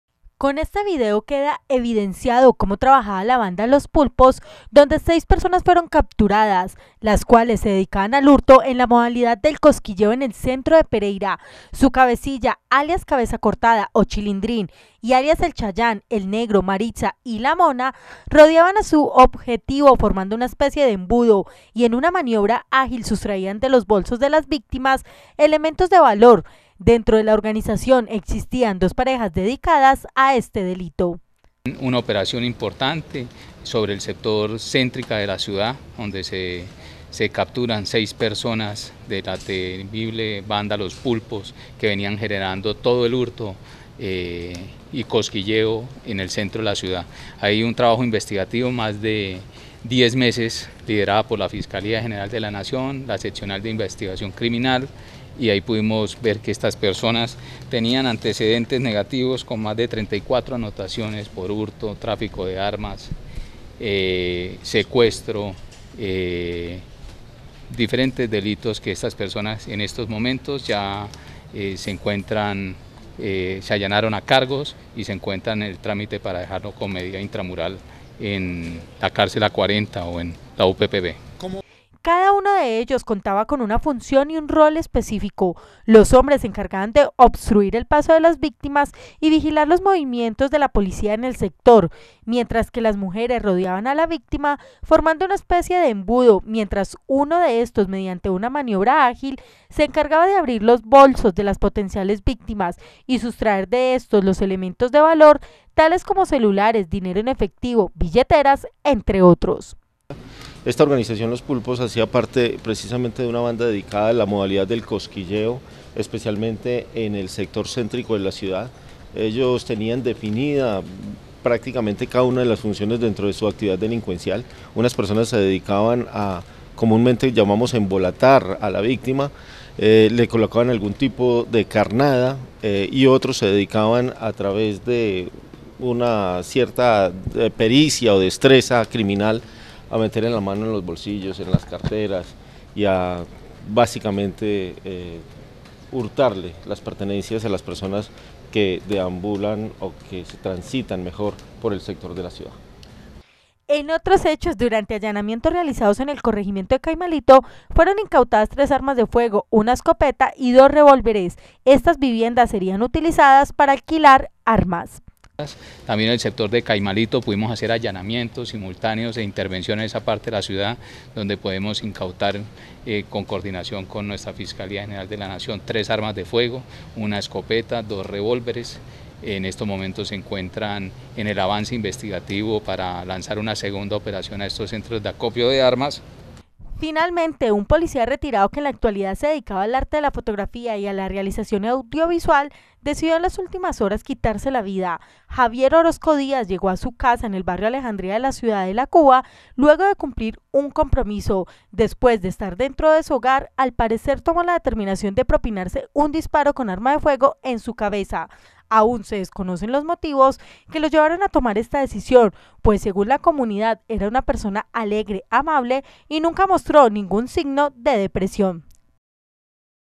LOS PULPOS, BANDA DE COSQUILLEROS DEL CENTRO DE PEREIRA.
LA DESARTICULACIÒN DE LA BANDA LOS PULPOS, MUJERES Y HOMBRES QUE SE DEDICABAN AL HURTO BAJO LA MODALIDAD DE COSQUILLEO EN EL CENTRO DE PEREIRA Y EL SUICIDIO DE UN POLICÍA RETIRADO DE RISARALDA QUE EN LA ACTUALIDAD SE DEDICABA A LA FOTOGRAFÍA, HACEN PARTE DE ESTE RESUMEN JUDICIAL DE INICIO DE SEMANA.